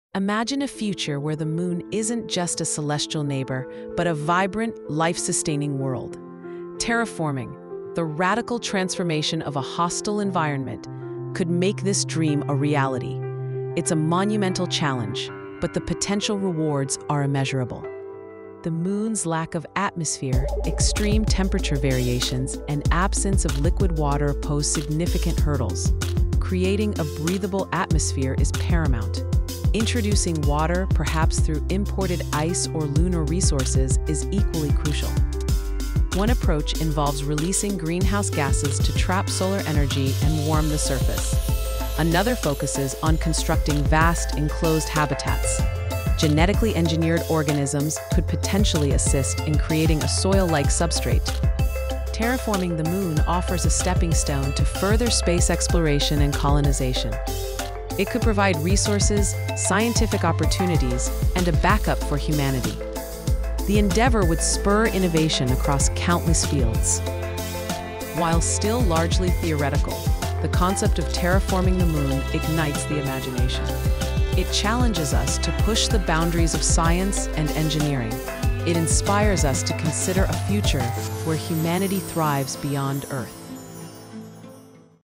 Terraforming the Moon: Exploring Humanity's Boldest Vision 🌕
Join us on a journey to explore the bold vision of terraforming the moon in this captivating video. Discover the possibilities and challenges of transforming the moon into a habitable planet for humanity. 🌕